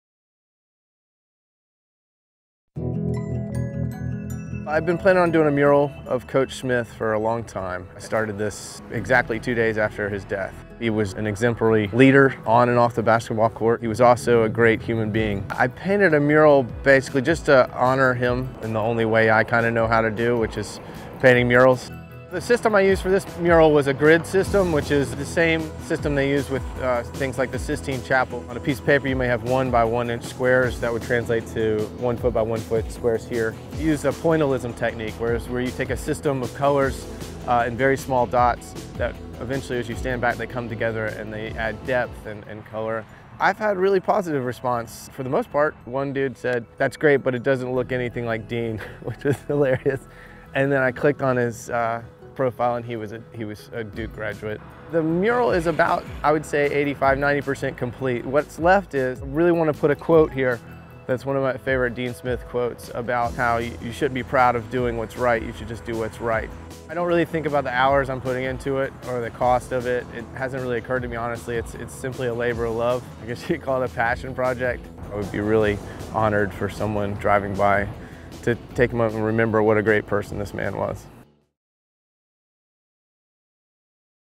A passion project
Carolina alum Scott Nurkin is close to completing his mural of Hall of Fame Basketball Coach Dean Smith – which Nurkin began two days after Smith’s death in February 2015. A look at the progress on this “passion project,” which can be viewed in person in Chapel Hill at the intersection of Smith Level Road and 15-501. Photos courtesy of B. Rain Bennett